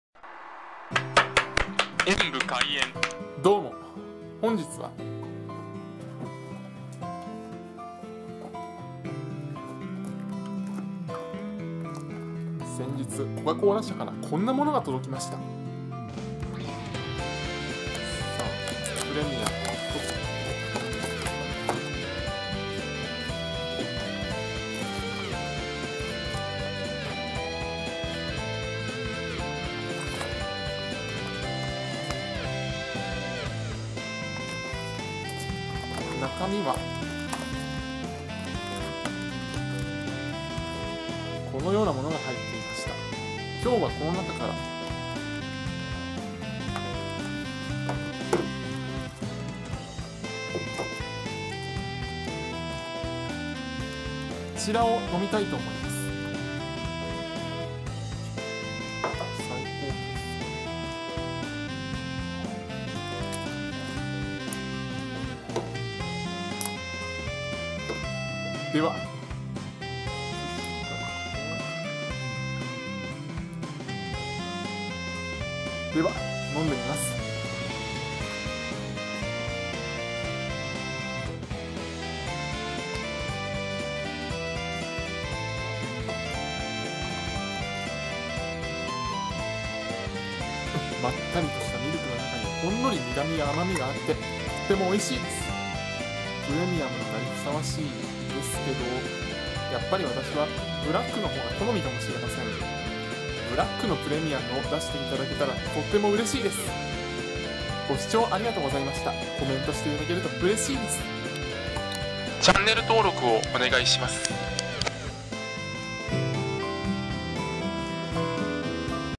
赤い彗星からの贈り物 GEORGIA プレミアム缶コーヒーを飲んでみた
GEORGIAプレミアム缶コーヒーを飲んでみました。
私の所に届けて下さったのは赤い彗星様でした～